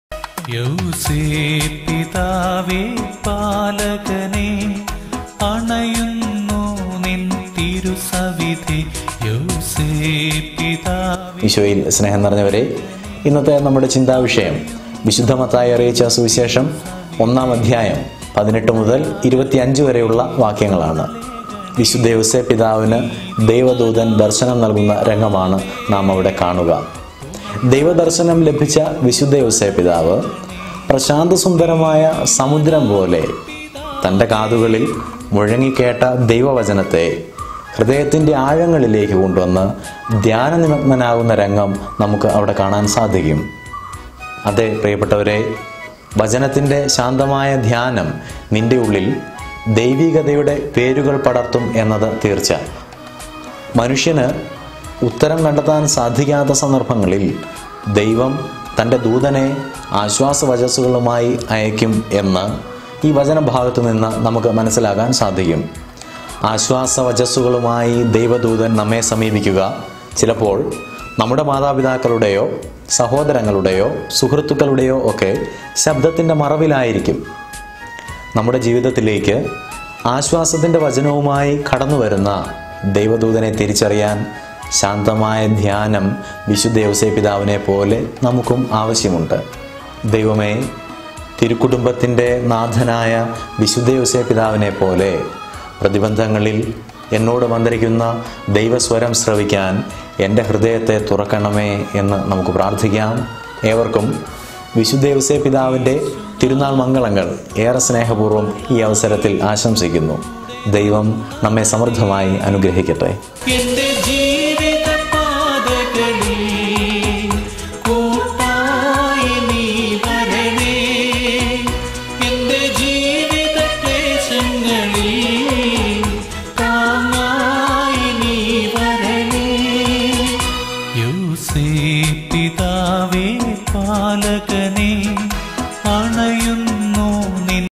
Fr. Aji Puthenpurackal. Short Thought. March 19, St. Joseph's feast. Mathew 1: 18-25
19 March 2019. Lenten Thoughts